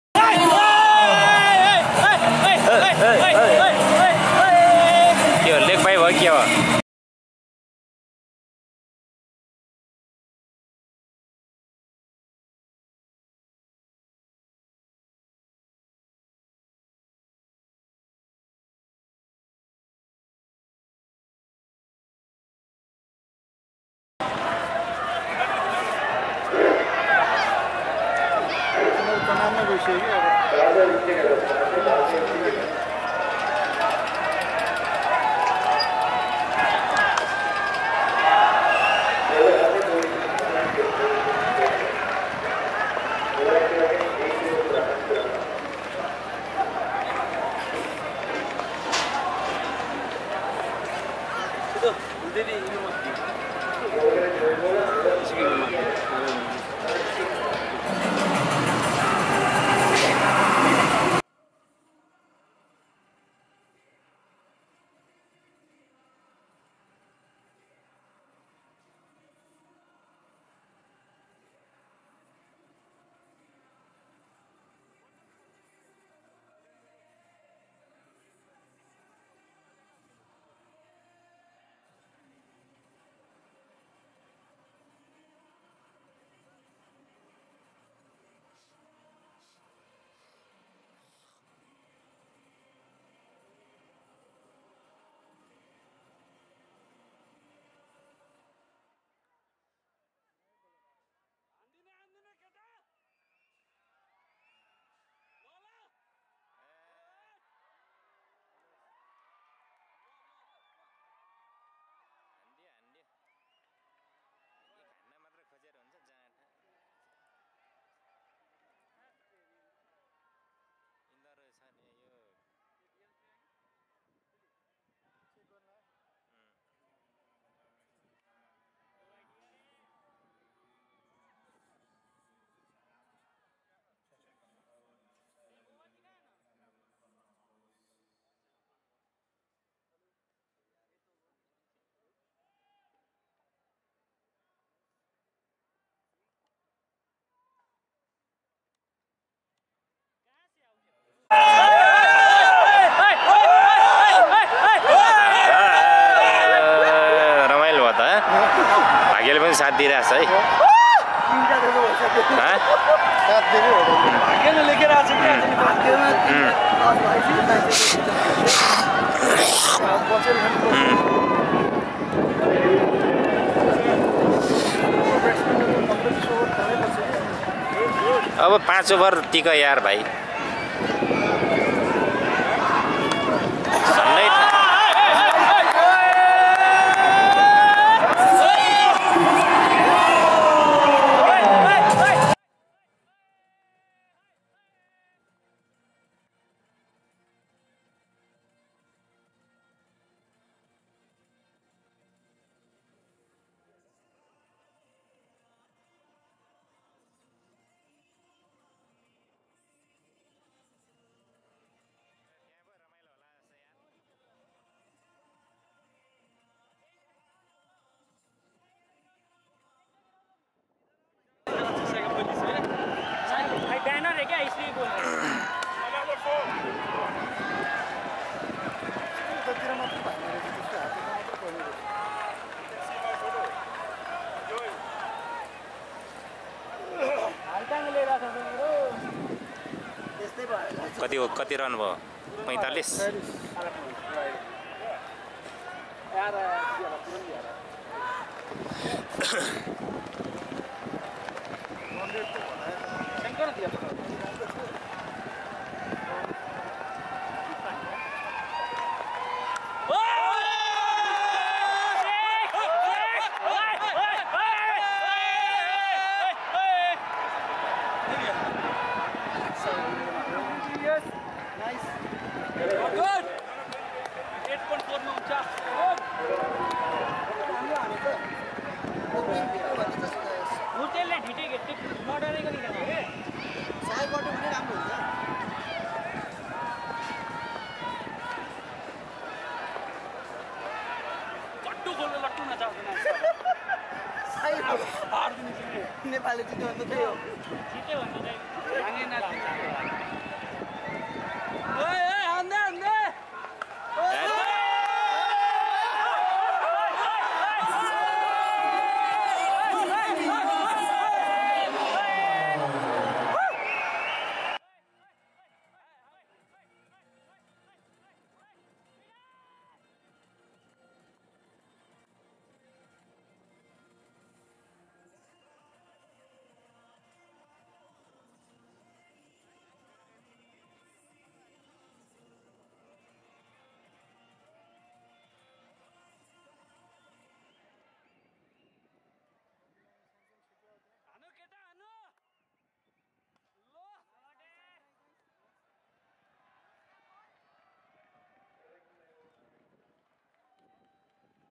Kushal and Aasif both hitting four on same over of Hardik Pandya | Bhurtel lifeline in DRS review
This is the moment when Field Umpire adjudge Kushal Bhurtel LBW out on the balling of Hardik Pandya. Kushal Went for DRS Review, where the earlier decision was ruled out. Afterwards, Both Kushal Bhurtel and Aasif Sheikh hit fours on the same over of Hardik Pandya.
Location Koteshwor
Copyright@AdamVlog